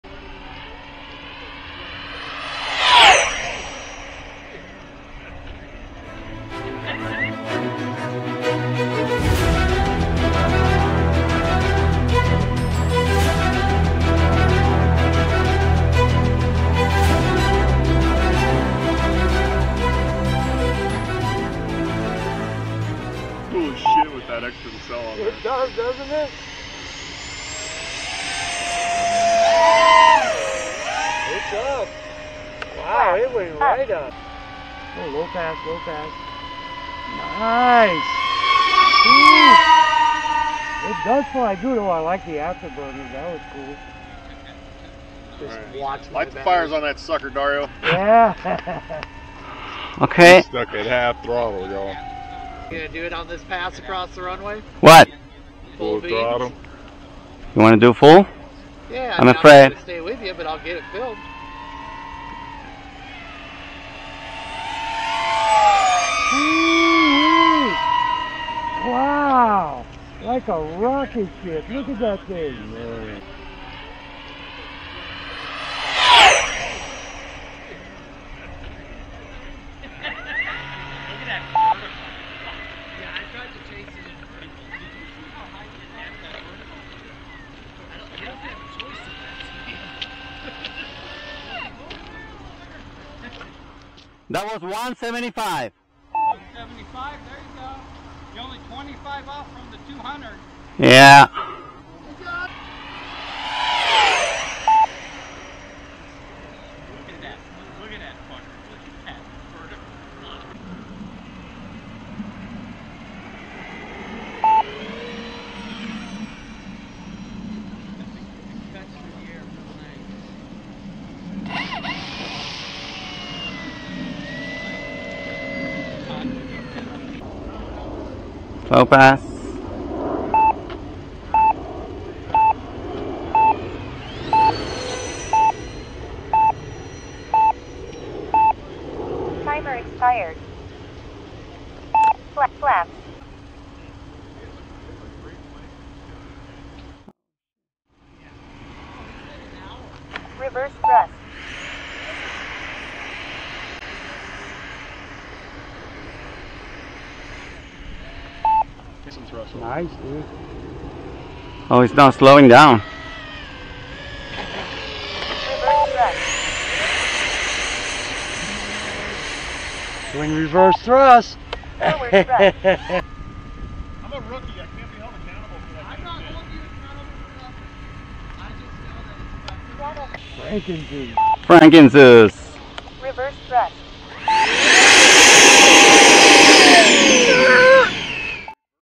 Freewing Zeus (Franken) 90mm 8s + 80mm 7s, built for speed
Maiden day of extreme mod to Freewing Zeus.
Removed vertical stabilizer and added a 3D printed 80mm nacelle with 2150kv inrunner running on 7s.
Main EDF is stock 8s.
Total thrust 7.5kg (16lbs).
Only a couple full throttle passes; maiden day was basically to test rates, CG, stall speed, flight characteristics and structural resistance.
Reached 175mph on level flight (maybe a slight dive).